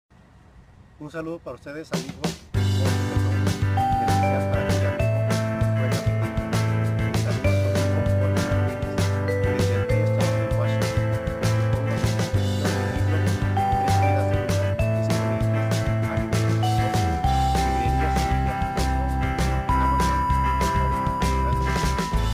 Saludos a los seguidores de Josue y LAS VOCES DE TODOS
Jorge Martinez el maravilloso escritor y guionista nos saludaaaa...GRACIAS GEORGY